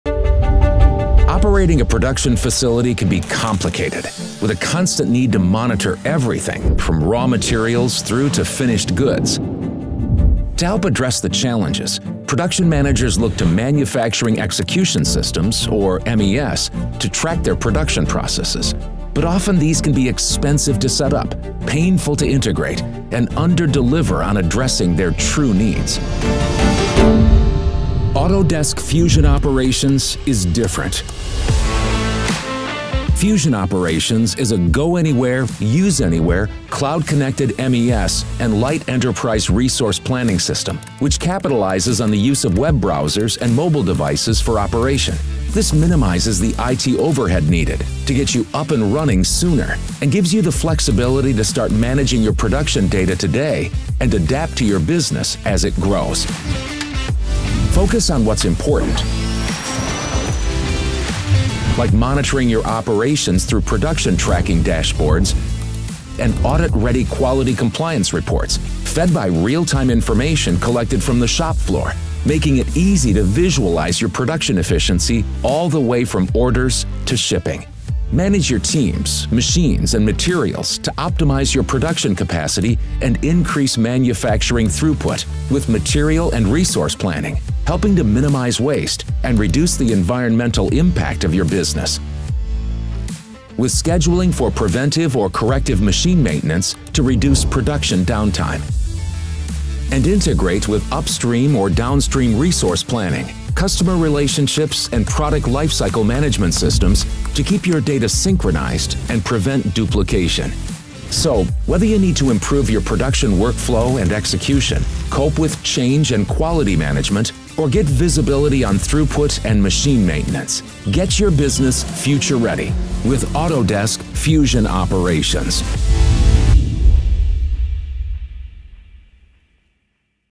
Production Planning Powered by Autodesk Fusion Operations
Production planning powered by real-time data. Manage your production process, eliminate unnecessary costs, and accelerate time to market with Autodesk Fusion Operations.